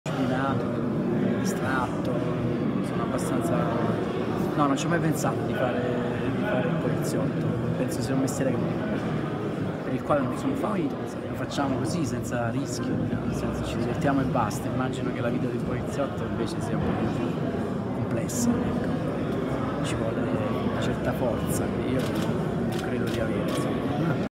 Intervista a Peppino Mazzotta, ispettore Fazio
L'intervista all'attore Peppino Mazzotta conosciuto come l'ispettore Fazio nella serie del "Commissario Montalbano".